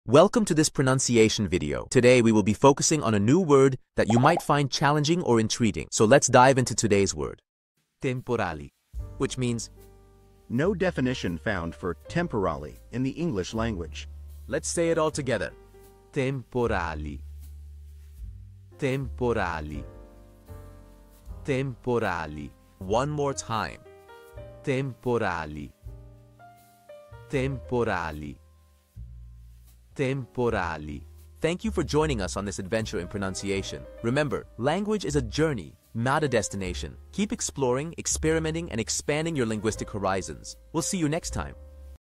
How to pronounce Temporali
Master the Pronunciation of 'Temporali Which means thunderstorms' - which means : No Definition Found for "Temporali" in The English Language. 📝 with @PizzaPronunciation 🎓🗣️ - [Your Guide to Italian]

Learn to pronounce 'Temporali' perfectly in Italian with our easy and engaging tutorial by @PizzaPronunciation! 📖📽️ In this comprehensive video, we provide you with native speaker examples 🗣️👂 and phonetic breakdowns 🎵 to ensure you get the pronunciation just right.

How to say Temporal in Italian